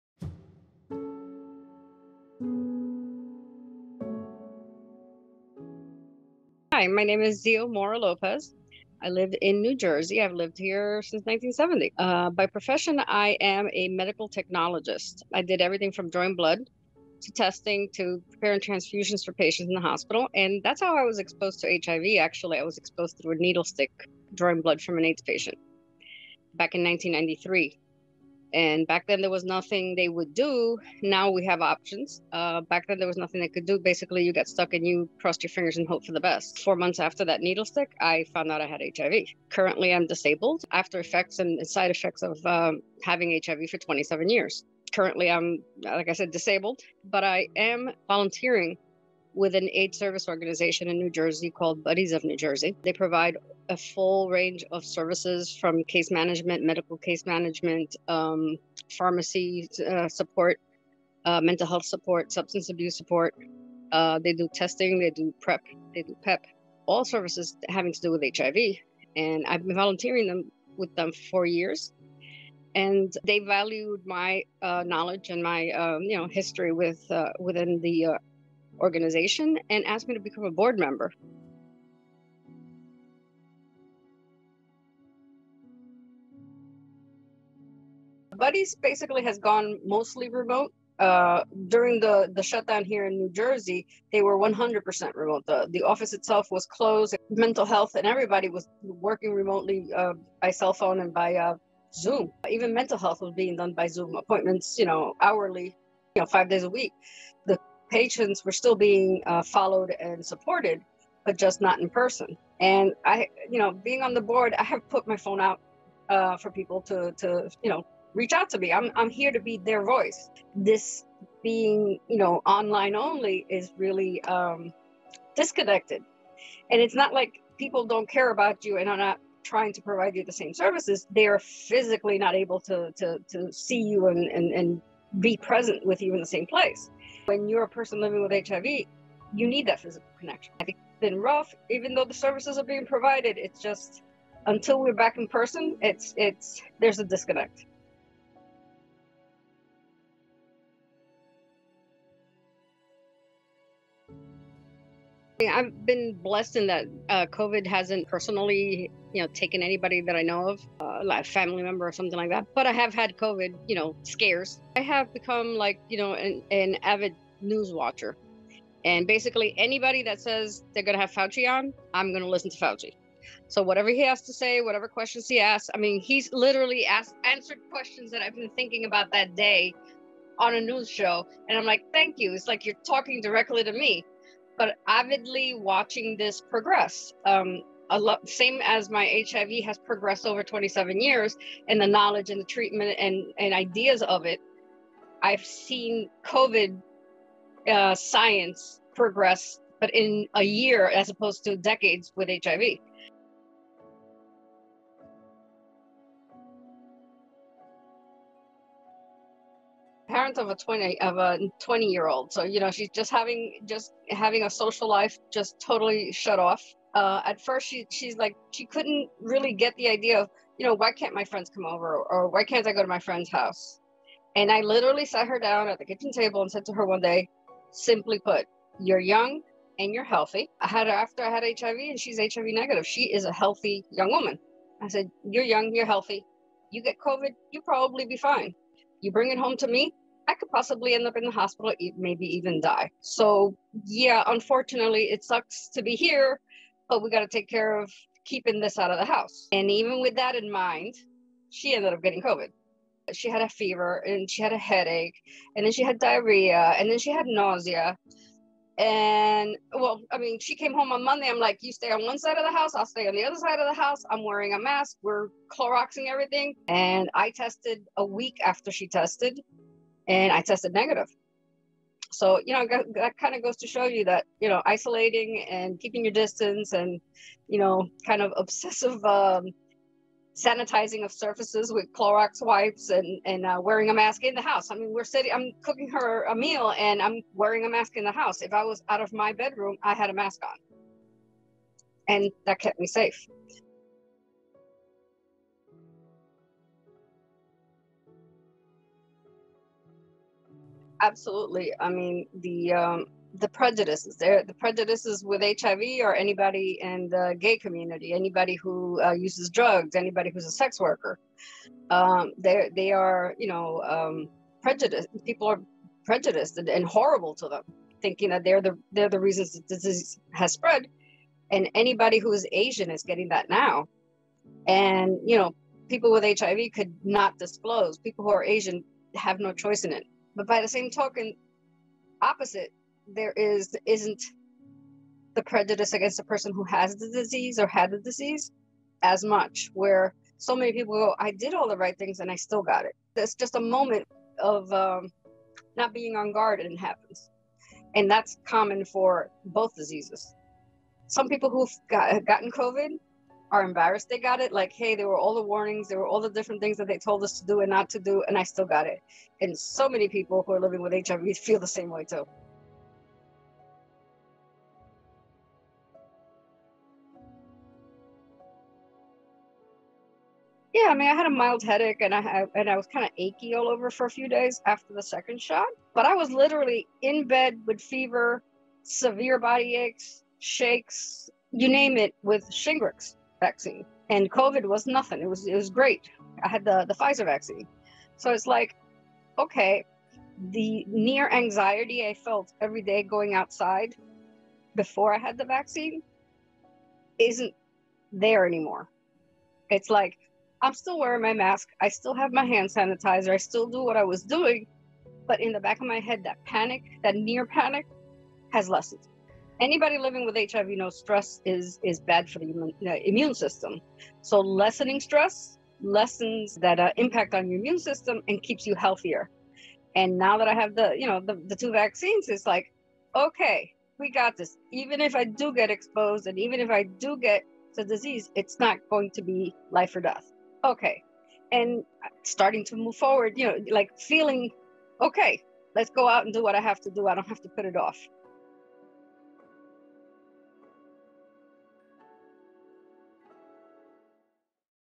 Connections episode 5 - Xio Mora Lopez
Xio Mora-Lope is a medical technologist, Board Member of Buddies of New Jersey, and a long-term survivor who contracted HIV after an accidental needle stick in 1993. In this episode, Xio shares her story, her insights, and her narrow escape from COVID-19.